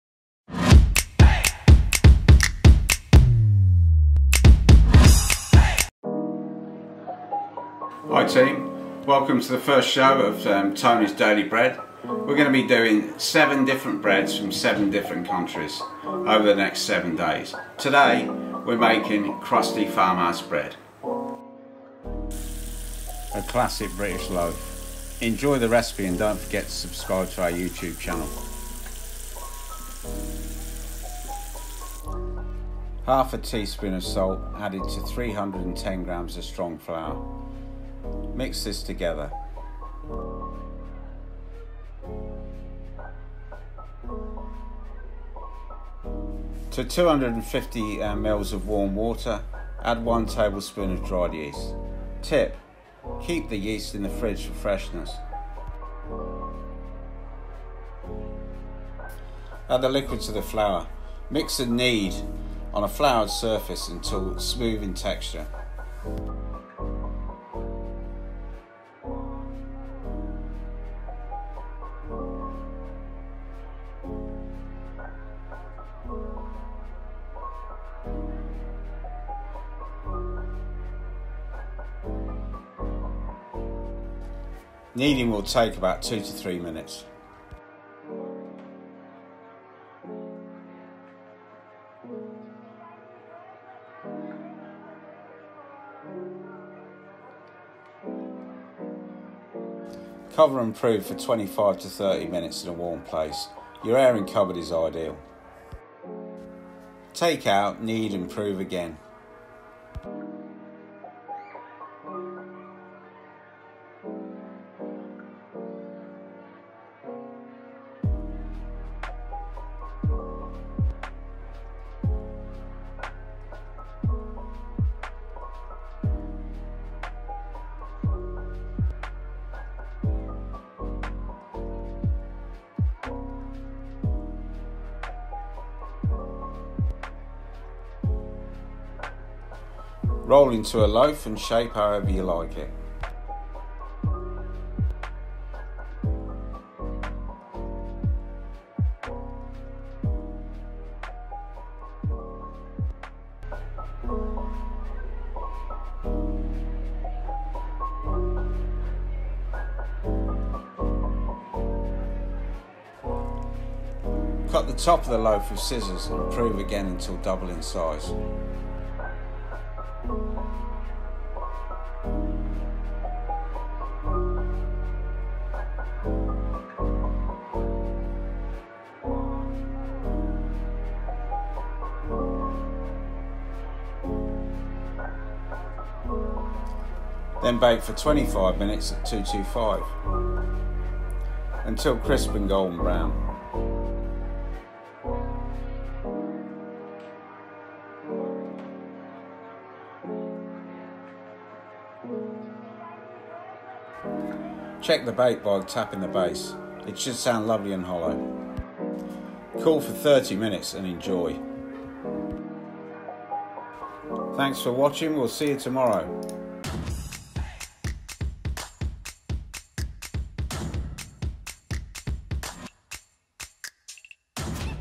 How To Make The Best Classic Farmhouse Loaf
Today from Great Britain:
 Crusty Farmhouse Bread made easy